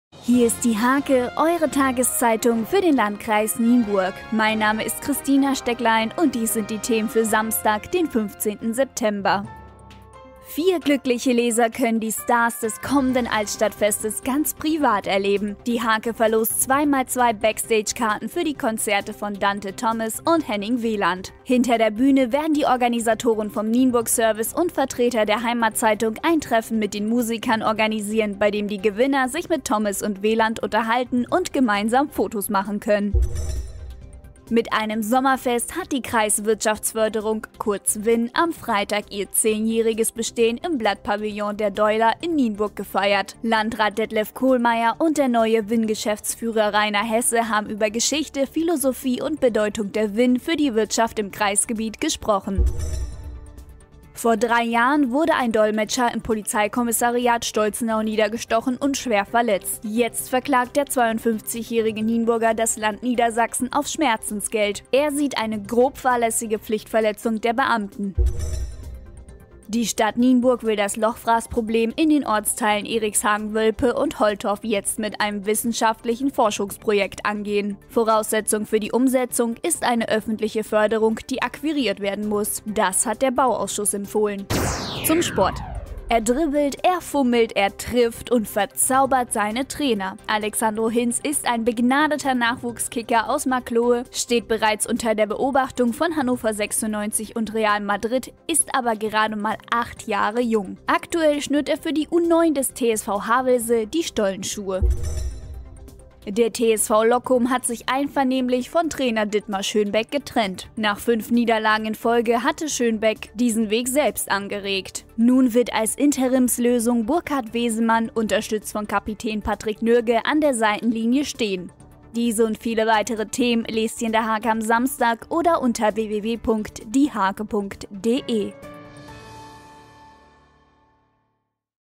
DIE HARKE vom 15.09.2018
In einer Woche wird zu dieser Zeit bereits fleißig beim Altstadtfest 2018 gefeiert - wen ihr da treffen könnt, seht ihr in der aktuellen Ausgabe der HARKE Video-News für Samstag, 15. September:

- Dante Thomas und Henning Wehland treffen
- Kreis-Wirtschaftsförderung feiert 10-Jähriges
- Dolmetscher verklagt Land auf Schmerzensgeld
- Forschungsprojekt zur Lochfraß-Problematik
- Begnadeter Nachwuchs-Kicker: Alexandro Hinz
- TSV Loccum trennt sich von Trainer Schönbeck